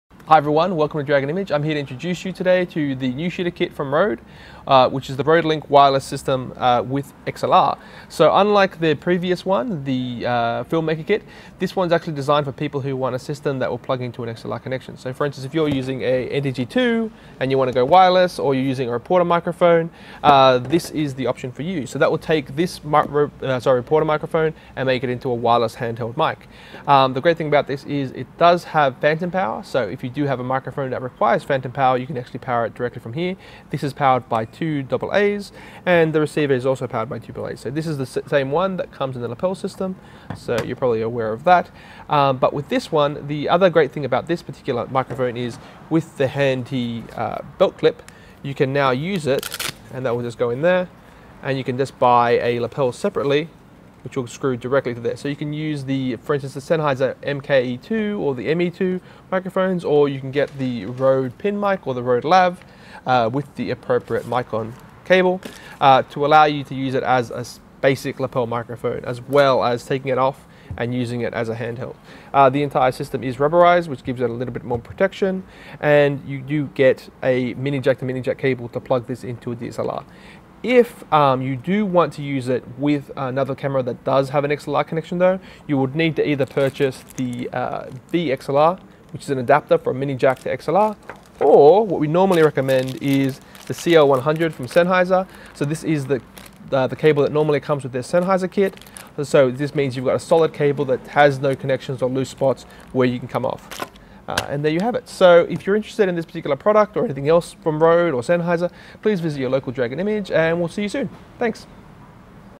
Need a wireless handheld adapter for your xlr mic? The RODE Newshooter kit could be the solution
Need a wireless handheld adapter for your xlr mic? The RODE Newshooter kit could be the solution. The system comes with an XLR Transmitter and a receiver for your camera and/or recorder. Because it is wireless, you no longer need XLR cables to get in the way and become trip hazards. Pairing it with a Sennheiser ME2 or MKE2 lapel, or a RODE lav lapel with Micon-1 Adapter, you can also use the system as a wireless lapel unit. This means you have 2 for the price of 1.
Perfect for interviews on location or in studio.